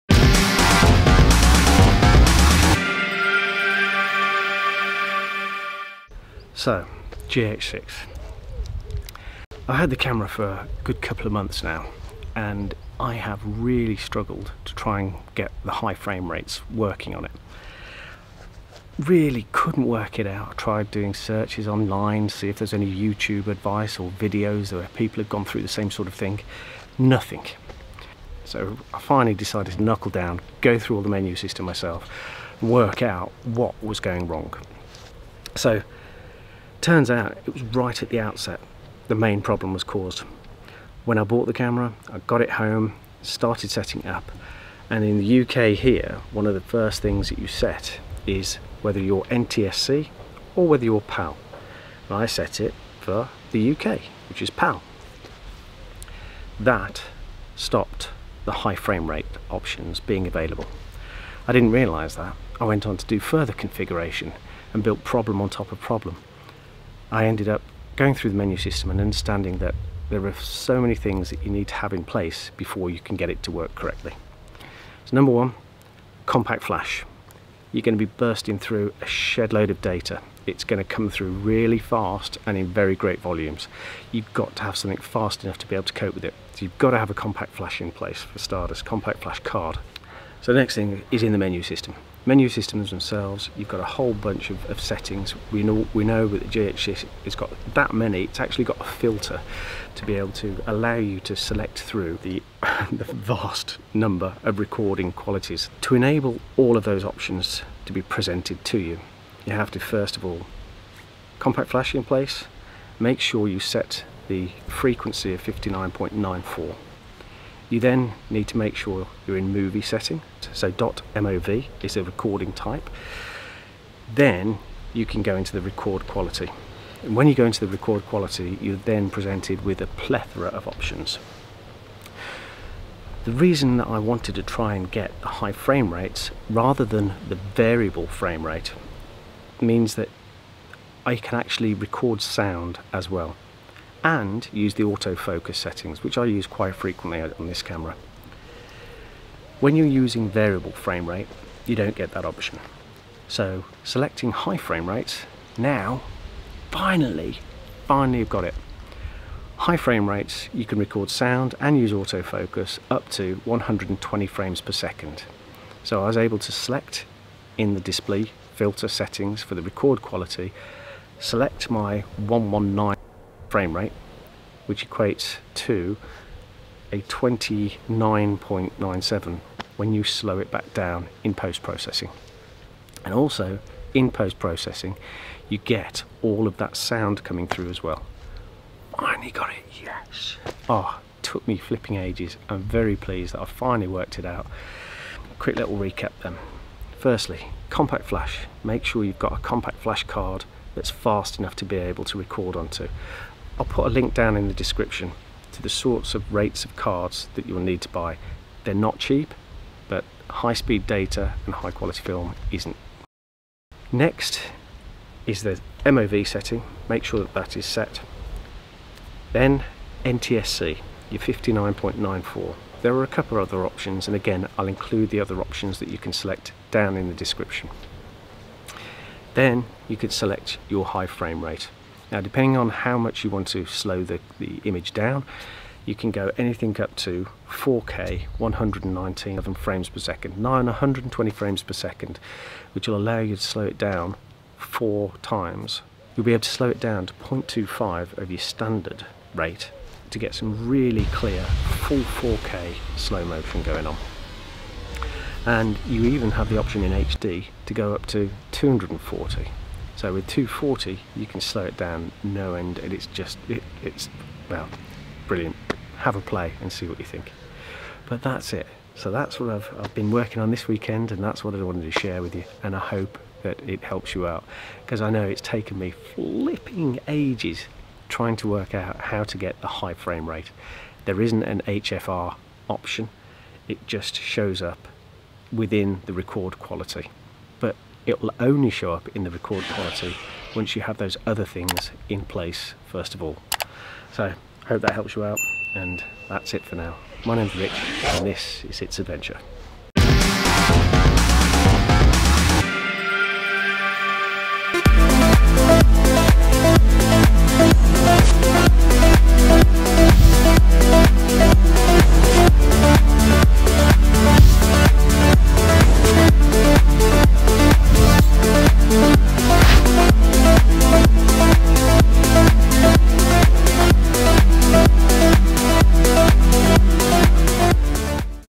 Got a GH6? This is the best Slow-Mo setting
I spent ages (and i do mean MONTHS) trying to get High Frame Rates working on my GH6!
I managed to get slow motion from use of 'Variable Frame Rate' VFR but that cut out the sound. So i wanted to uncover the 'HFR' - High Frame Rate setting. Spoiler. There isn't one!
What there is though is a set of Recording Qualities that have really high Frame Per Second values that can be slowed down in post processing.
This video goes into what i have found and how to achieve HFR recording!

1. Use a High Speed memory card - Compact Flash. I use this:
2. Turn off Dynamic Range Boost.
3. Set the 'System Frequency' to 59.94 'NTSC'
4. Set record mode to '.Mov'

The high frame rate settings available are here:

[System Frequency]              [Rec Quality]                                    Recording frame rate
                                                 [5.7K/48p/420/10-L]
                                                 [4.4K/48p/420/10-L]
                                                [C4K/48p/422/10-I(H)]
                                                [C4K/48p/422/10-I(L)]
                                                [C4K/48p/422/10-L]
                                                [C4K/48p/420/10-L]
 [59.94Hz (NTSC)]                [4K/48p/422/10-I(H)]                        47.95p
                                                [4K/48p/422/10-I(L)]
                                                [4K/48p/422/10-L]
                                                [4K/48p/420/10-L]
                                                [FHD/48p/422/10-I]
                                                [FHD/48p/422/10-L]
                                                [FHD/48p/420/10-L]

                                                [C4K/120p/420/10-L]
                                                [4K/120p/420/10-L]
                                                [FHD/120p/422/10-I]                       119.88p
                                                [FHD/120p/422/10-L]
                                                [FHD/120p/420/10-L]

                                                [FHD/240p/422/10-I]
                                                [FHD/240p/422/10-L]                      239.76p
                                                [FHD/240p/420/10-L]

Kit used:
Sony ZV E1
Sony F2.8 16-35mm
Sony F4 24-70mm
Sony ECM B1M Microphone

Sony ZV E1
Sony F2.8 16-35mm
Sony F4 24-70mm
Rode stereo Video Mic X
Zoom H6